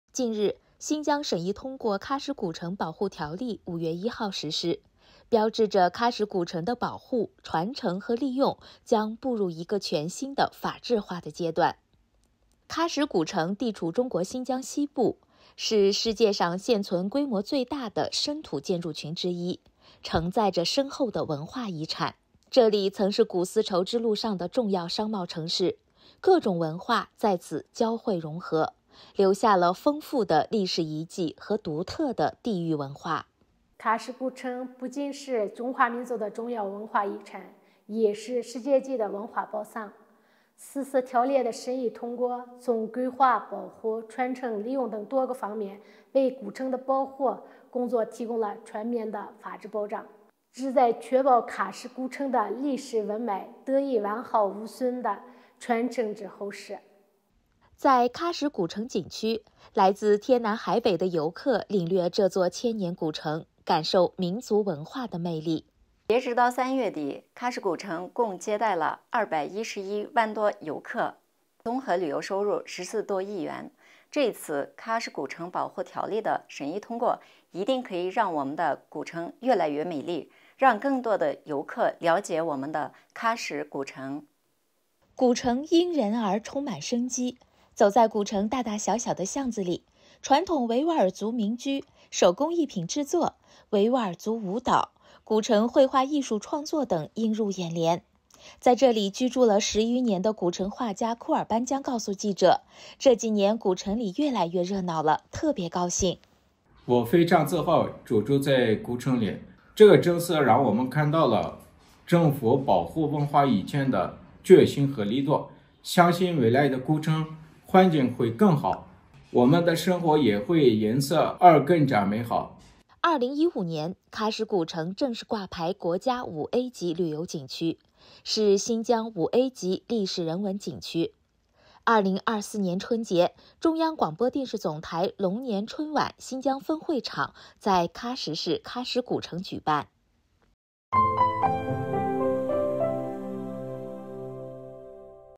新疆立法保护喀什古城 5月1日实施
近日，新疆审议通过喀什古城保护条例 5月1日实施，标志着喀什古城的保护、传承和利用将步入一个全新的、法治化的阶段。

喀什古城地处中国新疆西部，是世界上现存规模最大的生土建筑群之一，承载着深厚的文化遗产。这里曾是古丝绸之路上的重要商贸城市，各种文化在此交汇融合，留下了丰富的历史遗迹和独特的地域文化。

2015年，喀什古城正式挂牌国家5A级旅游景区，是新疆5A级历史人文景区。2024年春节，中央广播电视总台龙年春晚新疆分会场在喀什市喀什古城举办。（贾志鹏）